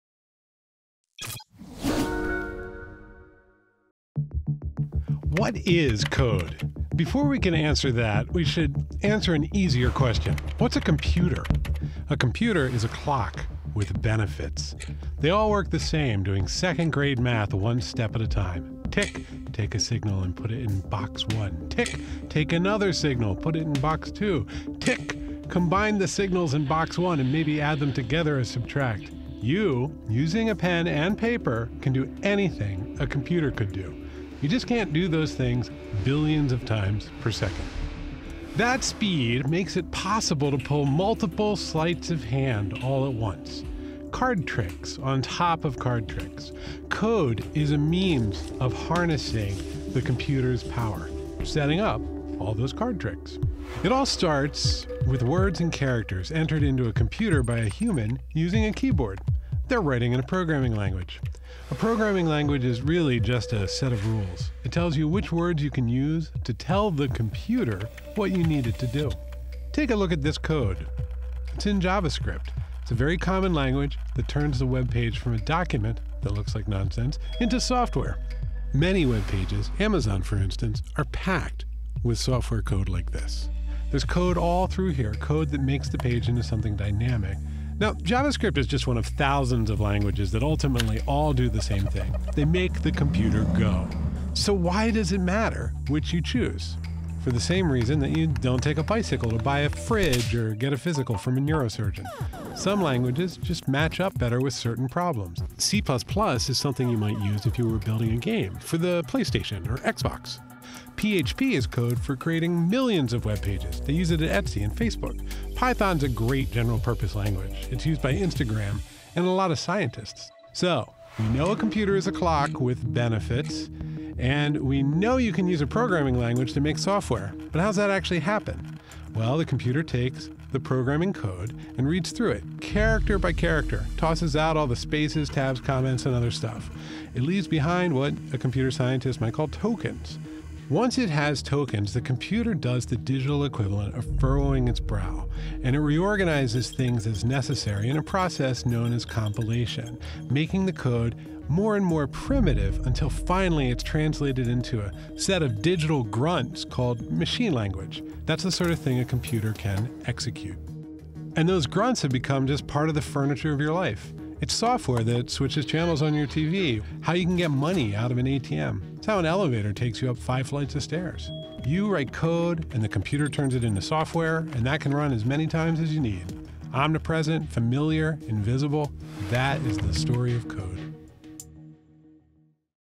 Invisible Computer Code Runs Your Life
June 10 -- We know that when we enter code into a computer we get software. And we know that software is part of the fabric of our lives - it switches channels on our cable boxes or spits out money from an ATM. But how does all that actually happen? Bloomberg Businessweek author Paul Ford explains. (Animation by Bran Dougherty Johnson.)